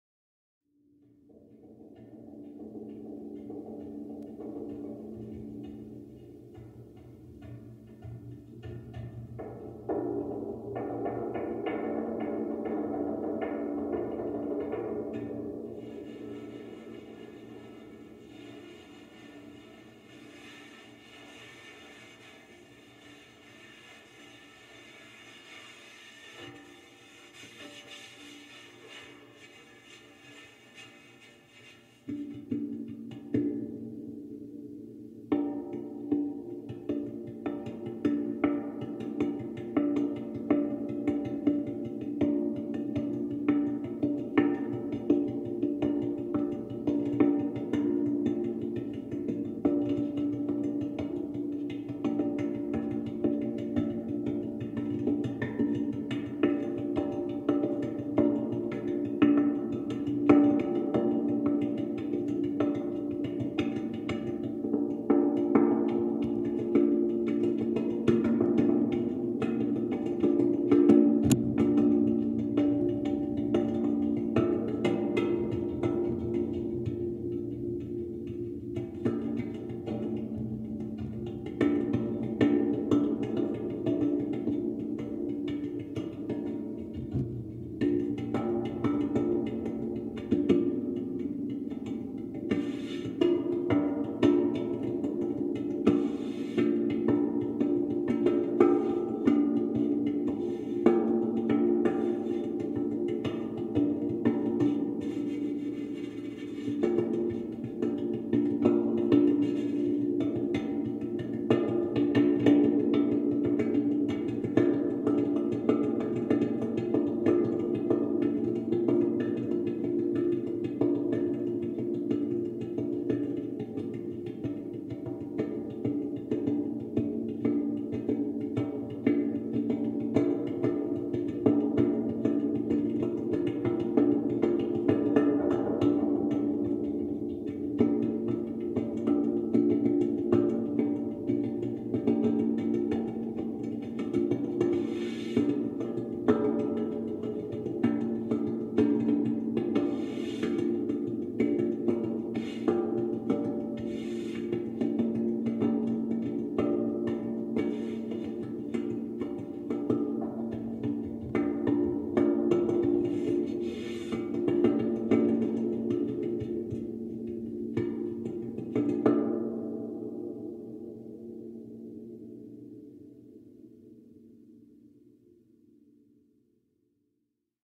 Mad Composer Lab: How to make music using an Electric Tower
This is an excerpt of the excitation of vibrations of an electric tower in the middle of the Mojave Desert.

It resonates around the pitch B flat.  I put a mic on it so you can hear the harmonics as well.